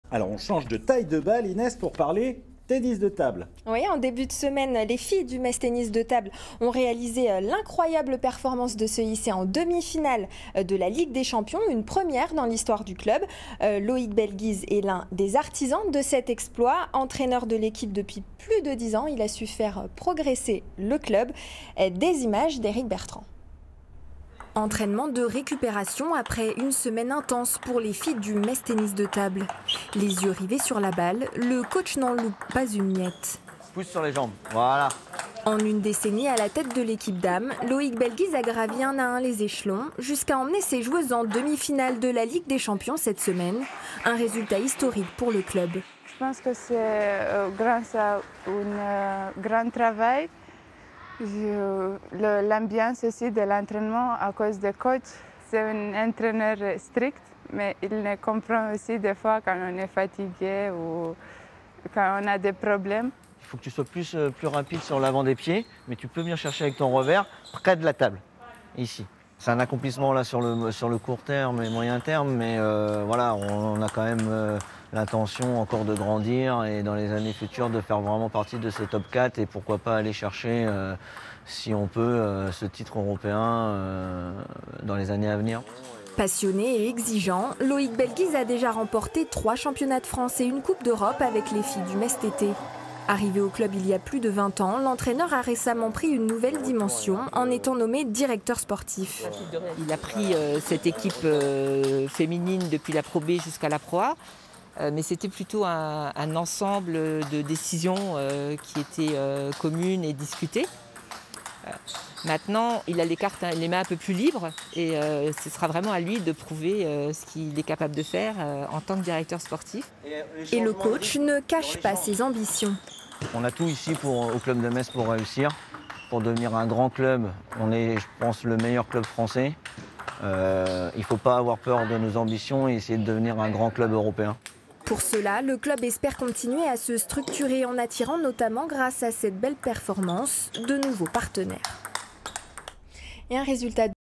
Tennis de table : Metz TT focus sur Loïc Belguise coach
METZ TT est en 1/4 de final en ligue des champions de tennis de table en 2020